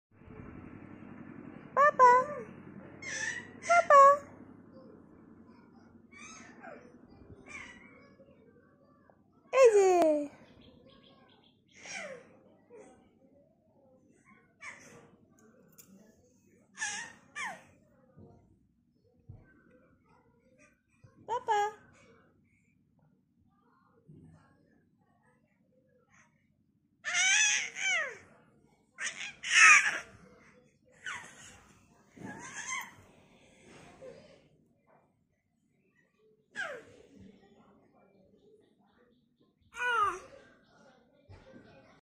Mayank aka aka upur hoye goppo korchhe Little Mayank vlogs
baby boy talking to his mother Baby boy talking to his mother